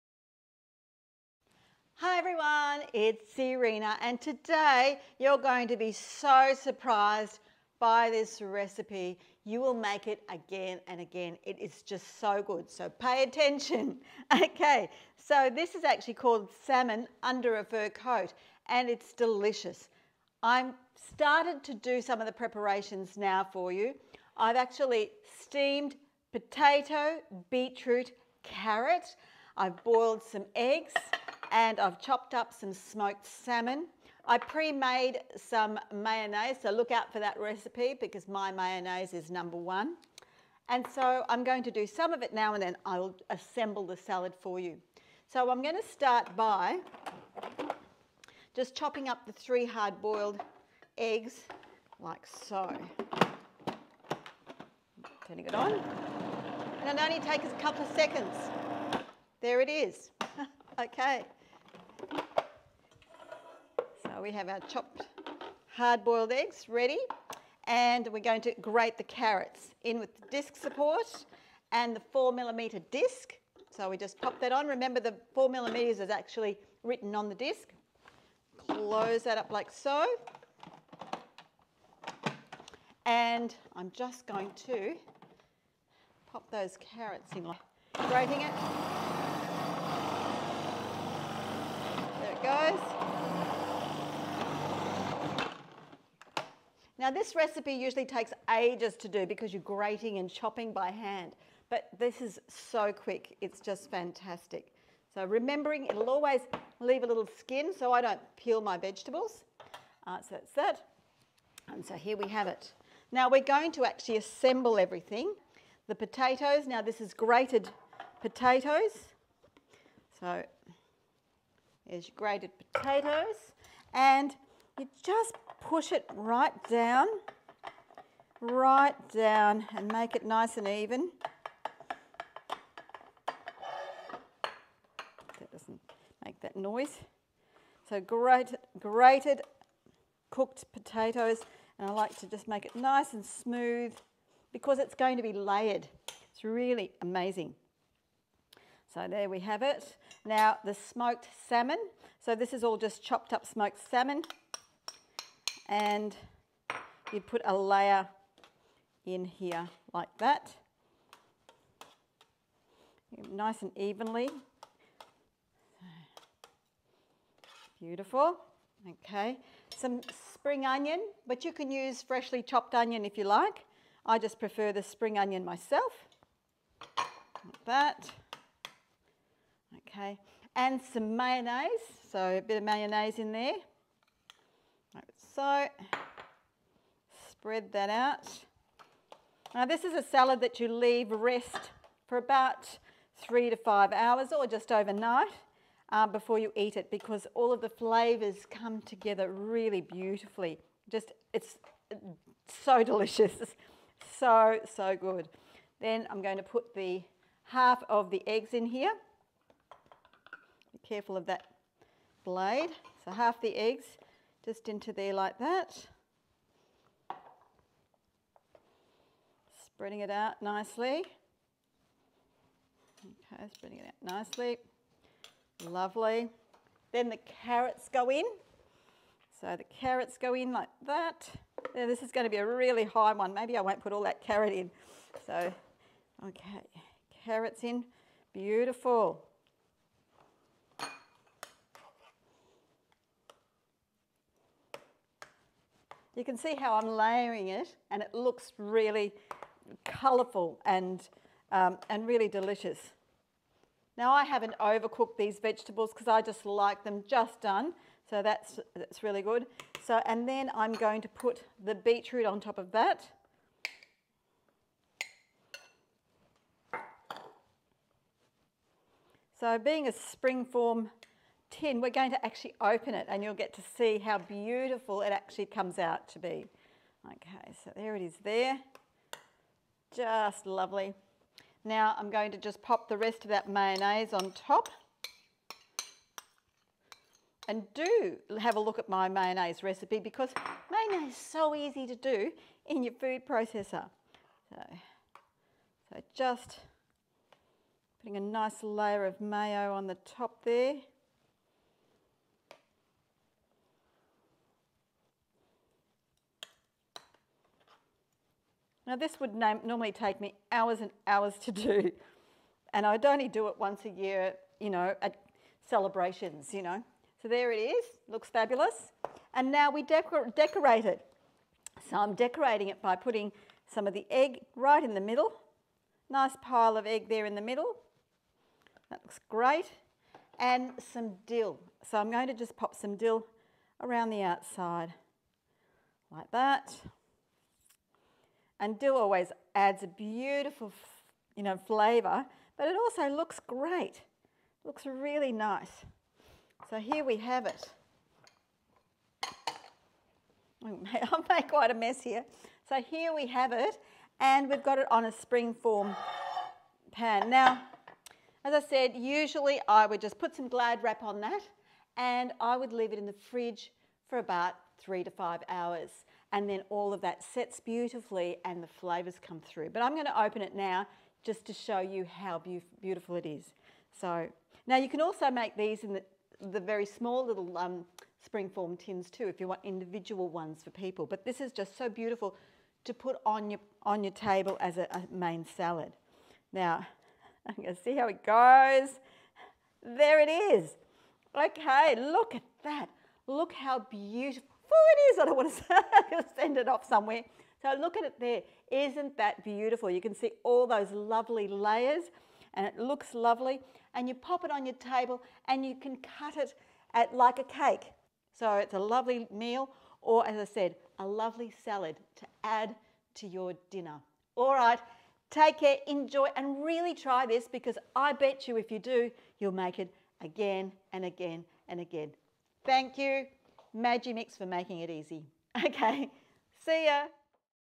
Salmon Under the Fur Coat Salad | Magimix 5 Minute Tips
In this video, Irena shows you one of her personal favourite salad recipes called Salmon Under the Fur Coat. A recipe that involves considerable food prep, having the right tools such as the Magimix Food Processor makes it quick and easy.

Ingredients:
500 g Smoked Salmon
3 large potatoes
2 large beets
2 carrots
2 cups mayonnaise
salt and pepper
3 hard boiled eggs
fresh dill chopped

Curious about a Magimix Product and want a one on one online demonstration?

SUBRSCRIBE to our channel for more product information and tips & tricks!